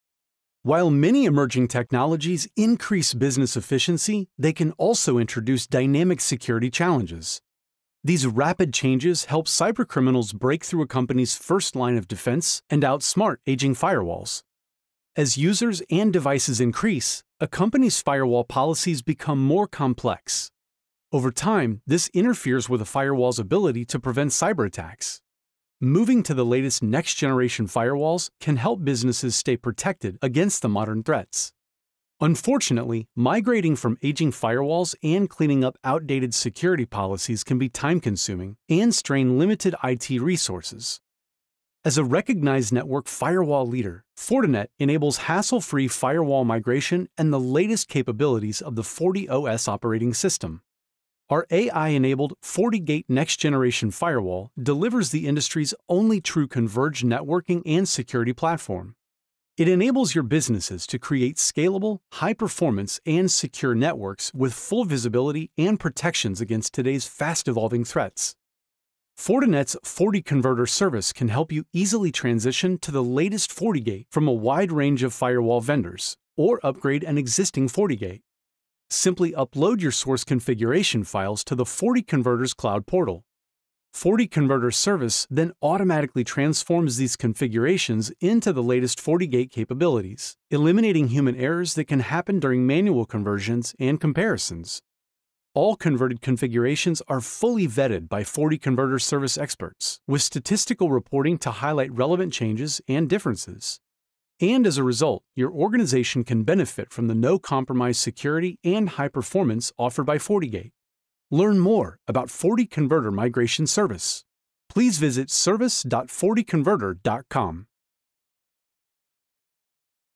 Migration to FortiGate NGFW Made Easy | FortiConverter Service
Fortinet's FortiConverter Service provides hassle-free migration to help organizations transition from a wide range of legacy firewalls to FortiGate Next-Generation Firewalls (NGFW) quickly and easily. This automated service simplifies the transition process by using proven migration methodologies that eliminate time-consuming effort and human errors from manual conversion. See how FortiConverter Service can help organizations move to FortiGate NGFW with confidence, and enjoy optimal network performance and security protection from the latest FortiOS technologies.